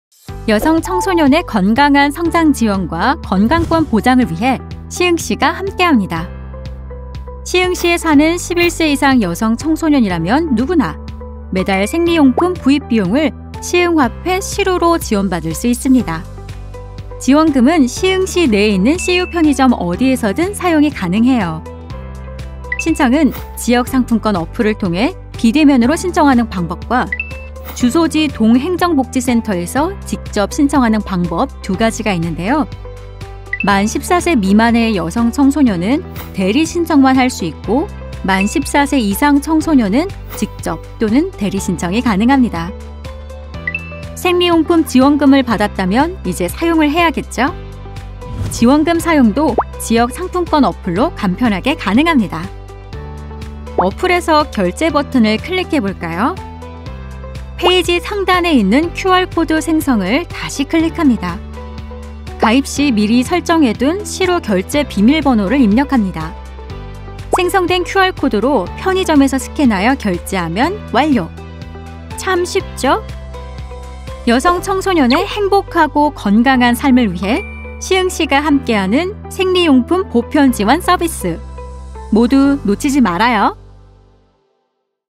[1분 시흥 사용설명서] 시흥시 여성청소년🙎🏻‍♀️ 생리용품 보편지원🎉
시흥시 내 주민등록이 되어있는 만 11세~18세 여성 청소년🙎🏻‍♀️을 위해
월 13,000원💰생리용품 구입비를 지원해 드립니다! (시흥화폐 시루로 지급)